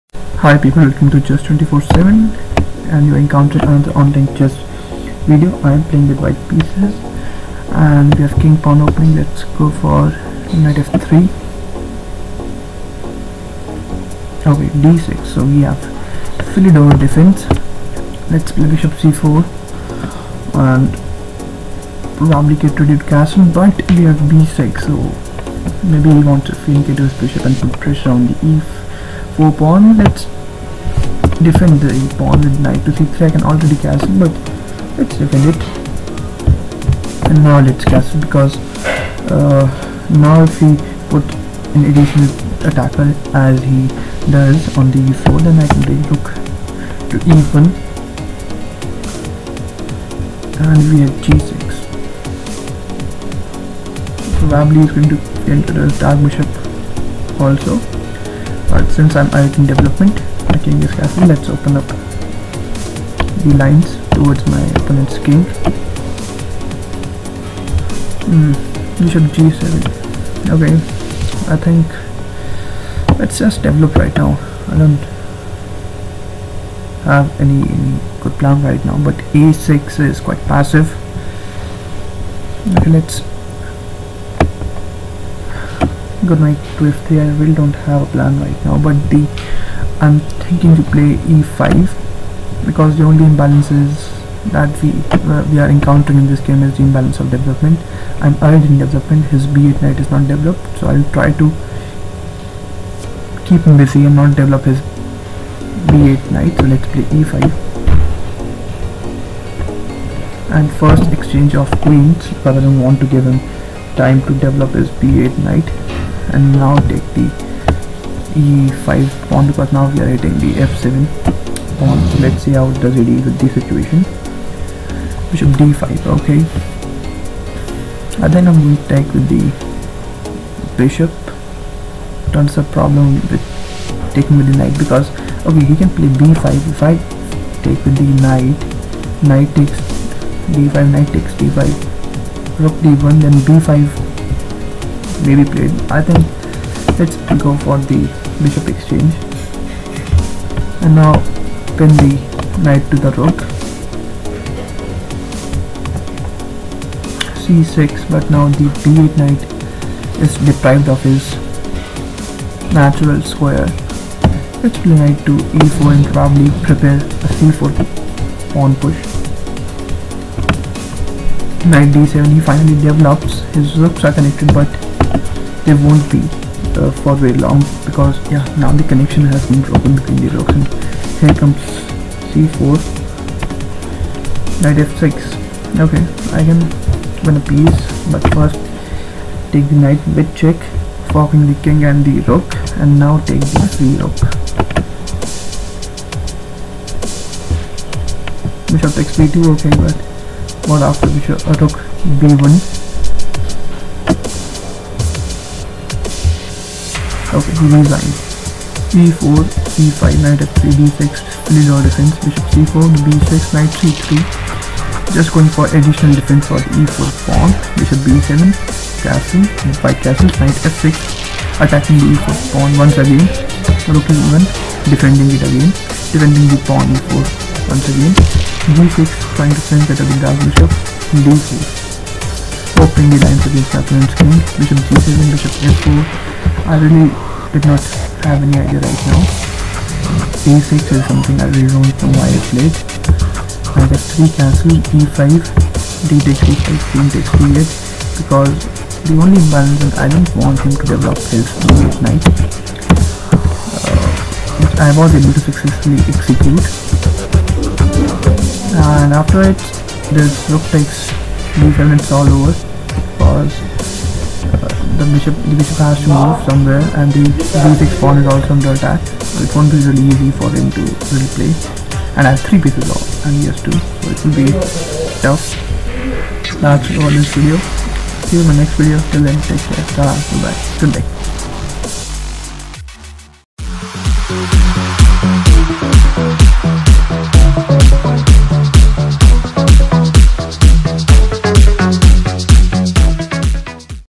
Chess 24X7 Game No. 39 Philidor's Defense ( C41 )
Hi people this is a live blitz game played on Fics (Free Internet Chess Server) I was playing with white pieces and went for e5. My opponenet replied with a philidor's defence. In my opinion it's a quite passive defence as black is not developing his pieces.

And even in the game black did not develop his b8 knight and I saw this as an opportunity to exploit as this was the only imbalance I could see easily.

The Philidor Defence is a chess opening characterised by the moves:

    1. e4 e5
    2. Nf3 d6

The opening is named after the famous 18th-century player François-André Danican Philidor, who advocated it as an alternative to the common 2...Nc6. His original idea was to challenge White's centre by the pawn thrust f7–f5.

Today, the Philidor is known as a solid but passive choice for Black, and is seldom seen in top-level play except as an alternative to the heavily analysed openings that can ensue after the normal 2...Nc6.

The ECO code for Philidor Defence is C41.

….▼▼ ▼FOLLOW ME▼▼▼ ….